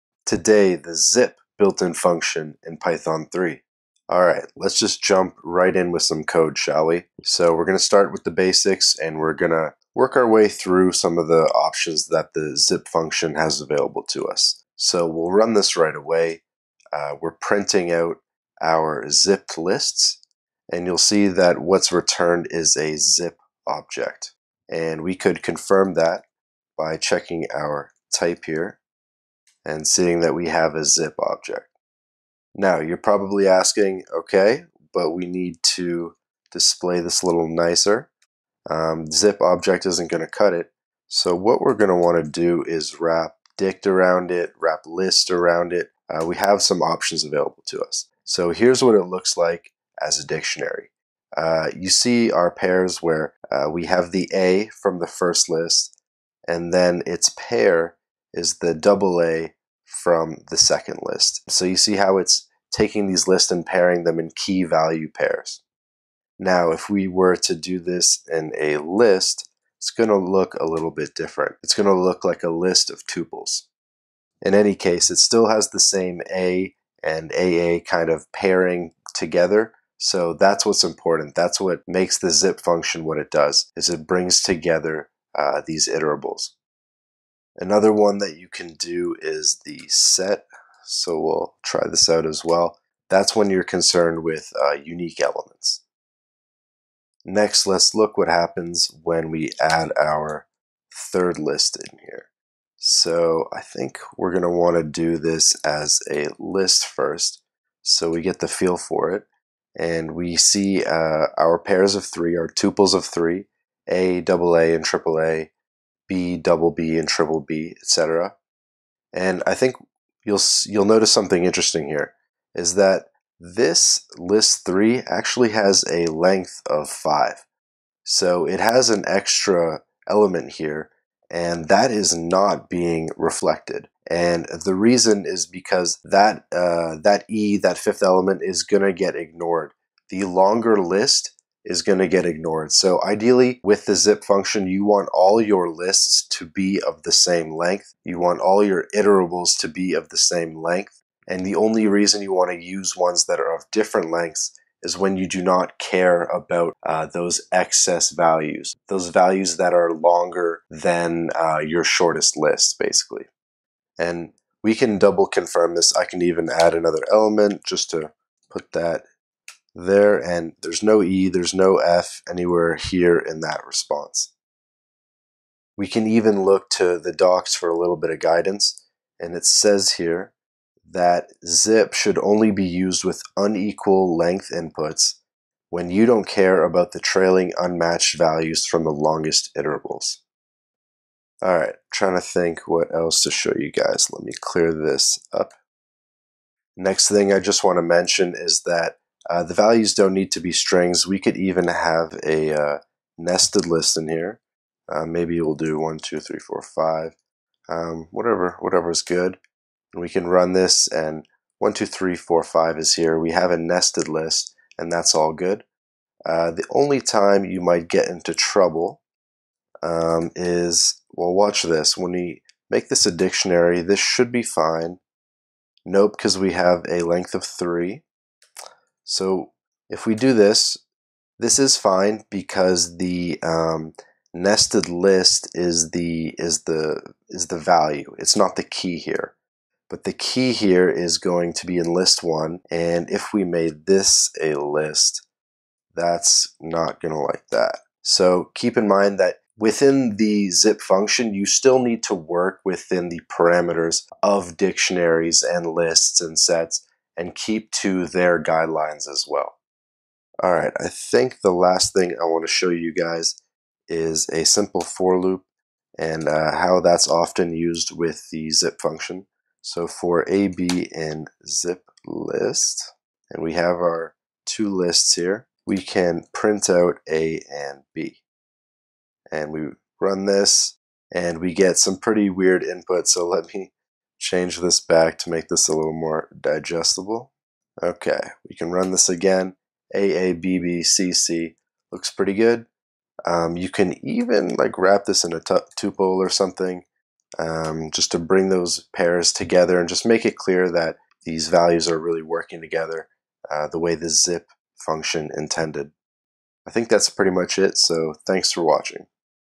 Python 3 zip() built-in function TUTORIAL
Tutorial on how to use the zip() built-in function from the Python 3 Standard Library.